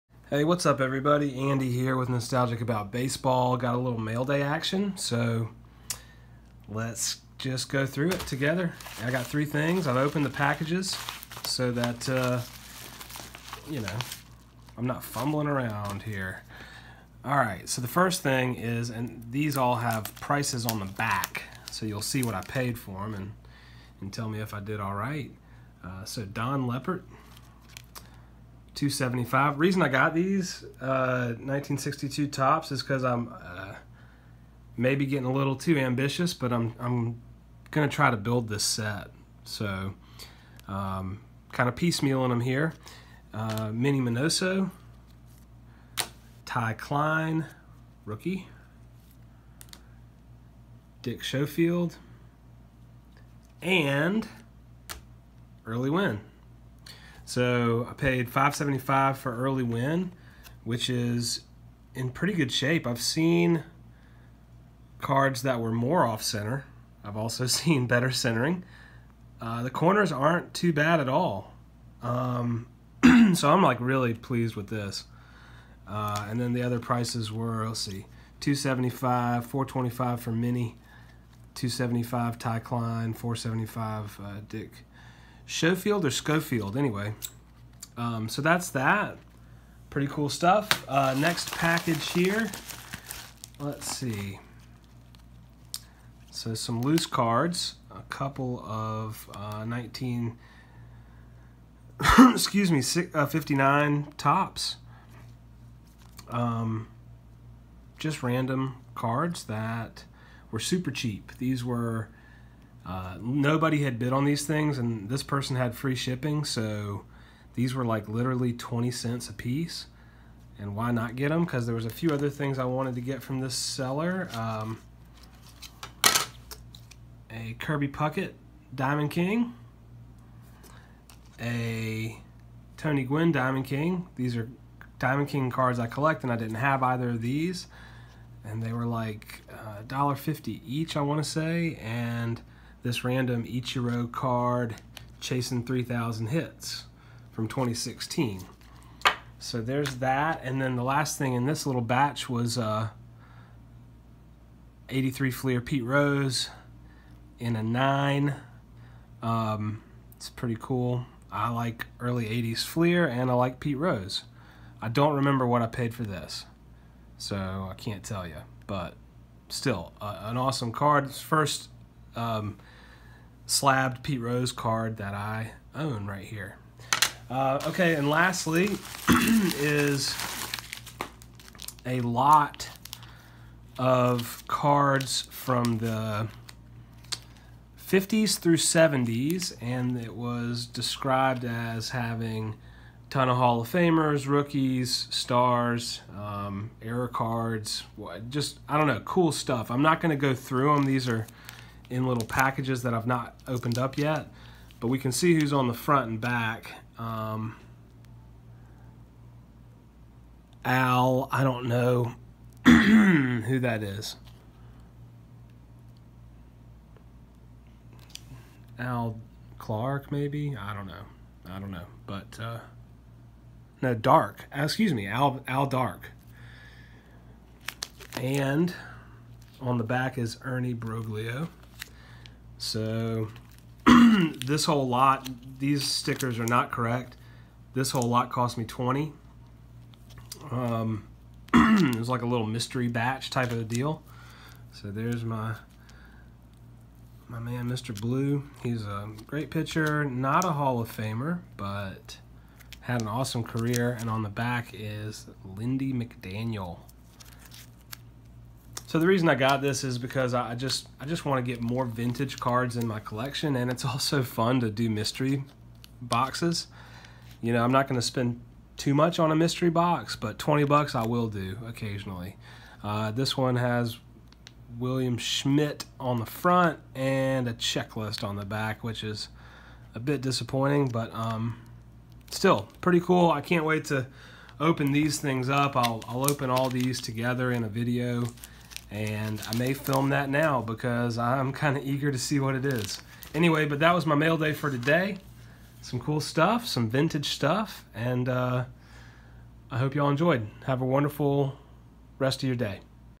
Mostly vintage eBay mailday
eBay mailday. Got some 1962 Topps, a slabbed Card, some diamond kings, and a few random cards.